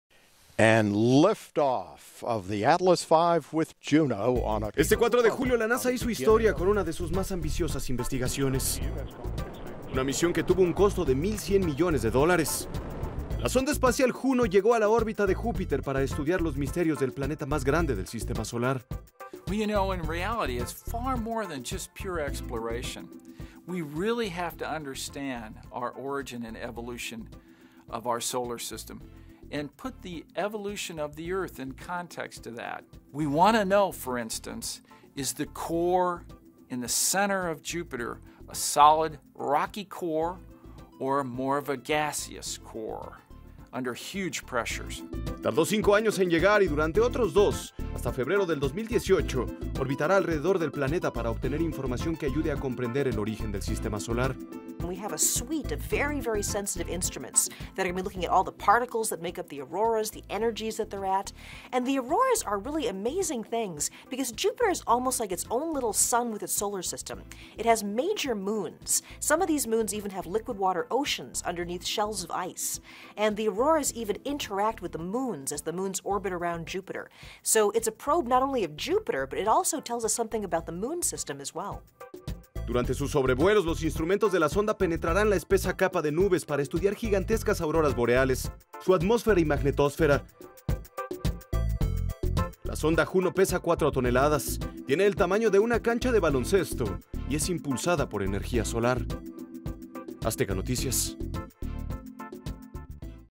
¿Qué es Juno?
La sonda espacial Juno pretende estudiar los misterios del planeta más grande del sistema solar: Júpiter.